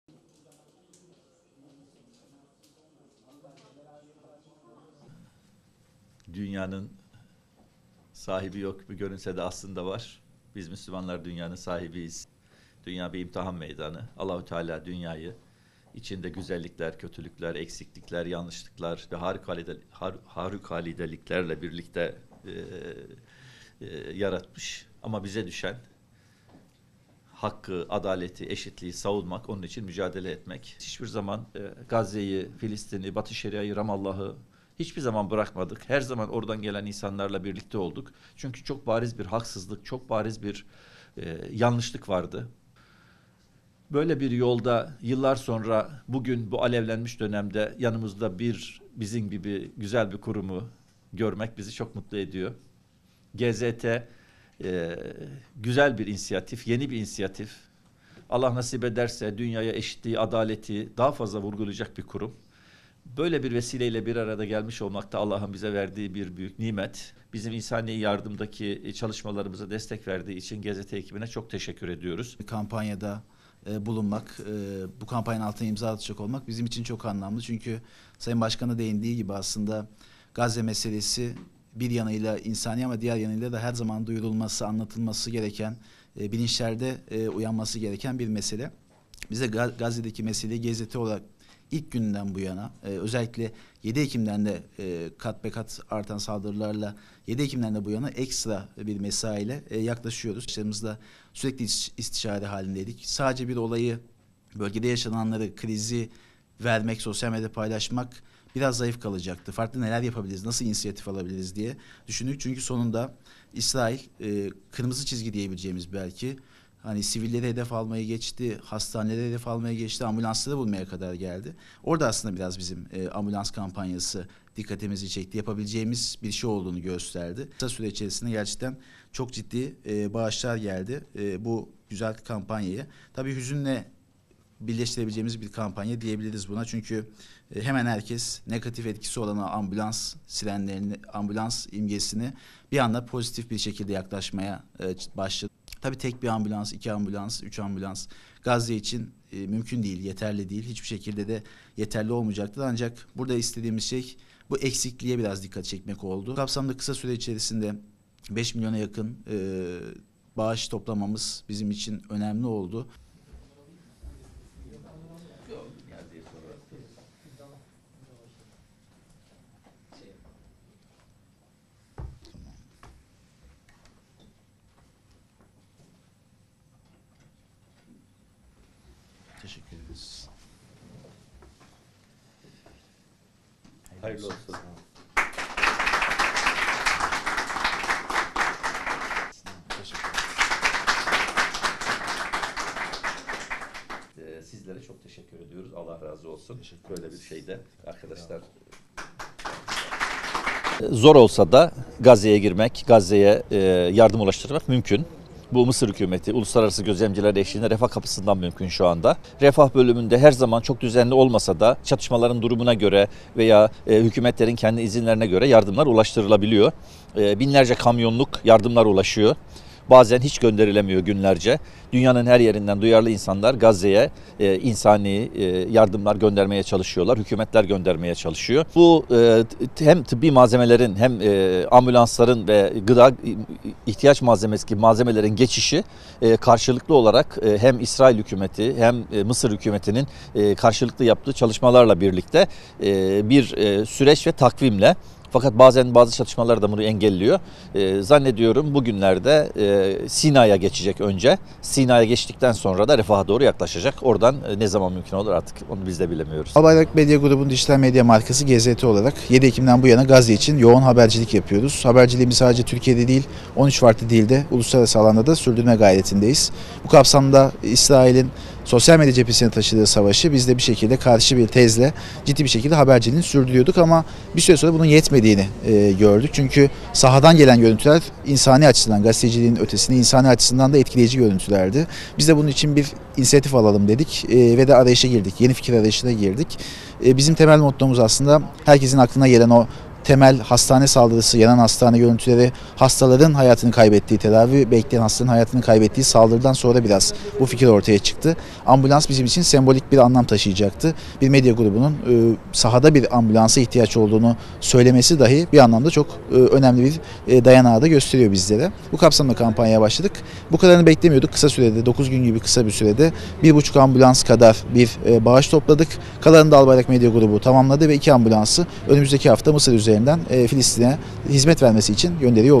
Gazze’ye Ambulans Projesi’nin iş birliği protokolü imzalandı
Gazze’ye Ambulans Projesi kapsamında 2 ambulans Gazze’ye doğru yola çıkıyor

İSTANBUL,(DHA)- Albayrak Medya Grubu Dijital Medya Markası GZT ve Yeryüzü Doktorları ‘Gazze’ye Ambulans’ kampanyası başlattı. Kampanya kapsamında 2 ambulans Gazze’ye gitmek üzere yola çıkacak ve oradaki vatandaşlara medikal malzeme desteği sağlayacak. Kampanyanın detayları düzenlenen basın toplantısıyla duyuruldu. Toplantıdan sonra Yeryüzü Doktorları Derneği Yönetim Kurulu Başkanı Uzm.

İsrail’in gerçekleştirdiği saldırılar neticesinde Gazze’de sağlık altyapısı çöktü ve hastaneler devre dışı kaldı. Bunun üzerine Albayrak Medya Grubu Dijital Medya Markası GZT ve Yeryüzü Doktorları ‘Gazze’ye Ambulans’ adında bir kampanya başlattı. Kampanya kapsamında 9 gün gibi bir sürede 5 milyon TL’den fazla bağış toplandı. Elde edilen bağışlarla Gazze’ye 2 ambulans ulaştırılacak.  Bugün Yeryüzü Doktorları Derneği Merkezi’nde düzenlenen basın toplantısında projenin detayları anlatıldı. Daha sonra Yeryüzü Doktorları Derneği Yönetim Kurulu Başkanı Uzm.

GÜNEY: BİZE DÜŞEN HAKKI, ADALETİ, EŞİTLİĞİ SAVUNMAK

İmza töreninde konuşan Yeryüzü Doktorları Derneği Yönetim Kurulu Başkanı Uzm. Dr. Yahyahan Güney, “Dünyanın sahibi yok gibi görünse de aslında var. Biz Müslümanlar dünyanın sahibiyiz.  Dünya bir imtihan meydanı, bize düşen hakkı, adaleti, eşitliği savunmak ve onun için mücadele etmek. Gazze'yi, Filistin’i ve Batı Şeria’yı hiçbir zaman bırakmadık. Her zaman oradan gelen insanlarla birlikte olduk. Çünkü çok bariz haksızlık ve yanlışlık vardı. Böyle bir yolda yıllar sonra bu alevlenmiş dönemde yanımızda bizim gibi güzel bir kurumu görmek bizi çok mutlu ediyor. GZT güzel ve yeni bir inisiyatif. Allah nasip ederse dünyaya eşitliği ve adaleti daha fazla vurgulayacak olan bir kurum. Böyle bir vesileyle bir araya gelmek de Allah’ın bize vermiş olduğu bir nimet. Bizim insani yardım çalışmalarımıza destek verdiği için GZT ekibine çok teşekkür ediyorum” dedi.

“REFAH KAPISINDAN MÜMKÜN”

Güney, “Zor olsa da Gazze’ye girmek ve yardım ulaştırmak mümkün. Bu şu anda Mısır hükümeti eşliğinde Refah kapısından mümkün. Refah bölümünde her zaman çok düzenli olmasa da çatışmaların durumuna göre veya hükümetlerin kendi izinlerine göre yardımlar ulaştırılabiliyor. Binlerce kamyonluk yardımlar ulaşıyor. Bazen ise günlerce yardım gönderilemiyor. Dünyanın her yerinden duyarlı insanlar Gazze’ye insani yardımlar göndermeye çalışıyor. Hem tıbbi malzemelerin hem de ambulans gibi ihtiyaçların geçişi karşılıklı olarak hem İsrail Hükümeti hem de Mısır Hükümetinin yaptığı çalışmalarla işliyor. Fakat bazen bazı çatışmalar bunu engelliyor. Zannediyorum ambulanslar önce Sina’ya geçecek. Sina’ya geçtikten sonra da Refaha doğru yaklaşacak” diye konuştu.

GEZER: BU SEMBOLİK BİR DURUM

GZT Genel Yayın Yönetmeni Doğukan Gezer ise “Bu kampanyada bulunmak bu kampanyanın altına imza atacak olmak bizim için çok anlamlı. Çünkü Sayın Başkan'ın değindiği gibi aslında Gazze meselesi bir yanıyla insani ama diğer yanıyla da her zaman bir duyurulması ve anlatılması gereken bir mesele. Biz de Gazze’deki meselelere GZT olarak ilk günden bu yana özellikle 7 Ekim'den beri katbekat artan saldırılarla ekstra bir mesaiyle yaklaşıyoruz. Arkadaşlarımızla sürekli istişare halindeydik. Farklı neler yapabiliriz? Nasıl insiyatif alabiliriz ? diye düşündük. Sonunda İsrail belki kırmızı çizgi diyebileceğimiz ambulansları vurmaya kadar geldi. Ambulans kampanyası dikkatimizi çekti. Yapabileceğimiz bir şey olduğunu gösterdi. Kısa süre içerisinde gerçekten çok ciddi bağışlar geldi. Bu güzel bir kampanya, tabi buna hüzünle birleştirebileceğimiz bir kampanya diyebiliriz. Gazze için bir, iki, üç  ambulans yeterli değil, hiçbir şekilde de yeterli olmayacaktır. Ancak burada istediğimiz şey bu eksikliğe biraz dikkat çekmek oldu. Bu kapsamsa kısa süre içerisinde 5 milyona yakın bağış toplamamız önemli oldu. Bu sembolik bir durum ancak bir duyarlılık olması açısından, medya grubunun bir şekilde bunu ön plana çıkarması, dünya genelinde konuşması önemli ve kıymetliydi. O yüzden bu fırsatı sağladığı için Yeryüzü Doktorlarına teşekkür ediyorum” dedi.

“2 AMBULANSI ÖNÜMÜZDEKİ HAFTA MISIR ÜZERİNDEN FİLİSTİN’E HİZMET VERMESİ İÇİN GÖNDERİYOR OLACAĞIZ”

Gezer, “Albayrak Medya Grubu Dijital Medya Markası GZT olarak 7 Ekim’den bu yana Gazze için yoğun habercilik yapıyoruz. Haberciliğimiz sadece Türkiye’de değil 13 farklı dilde uluslararası alanda da sürdürme gayretindeyiz.